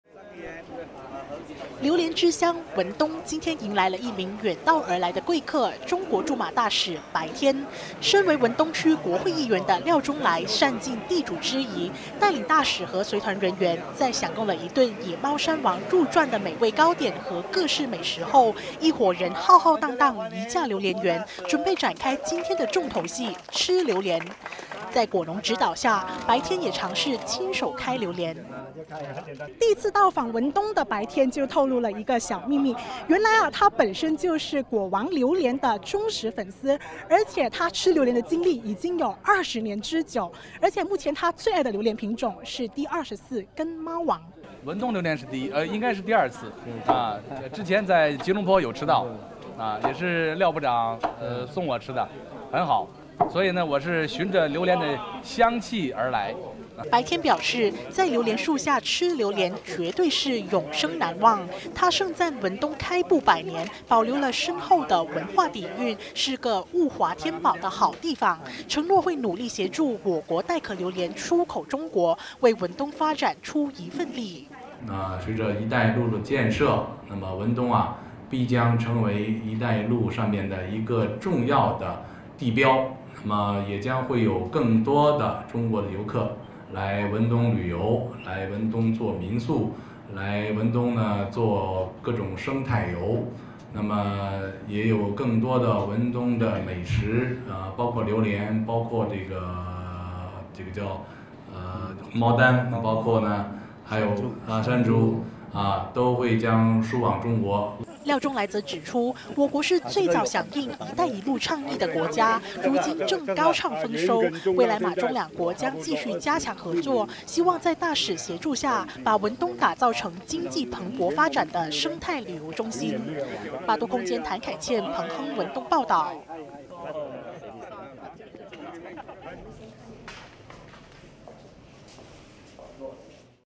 中国大使最爱吃榴梿 白天愿助带壳榴梿进神州
第15任中国驻马大使白天今天在马华总会长拿督斯里廖中莱的盛情邀约下，到访彭亨文冬。初次到访榴梿之乡的白天原来是一个资深榴莲发烧友，除了对王中之王猫山王赞赏有加，也承诺会协助我国带壳榴梿出口到中国，协助我国果农赚取更多外汇，并促进文冬的生态旅游发展。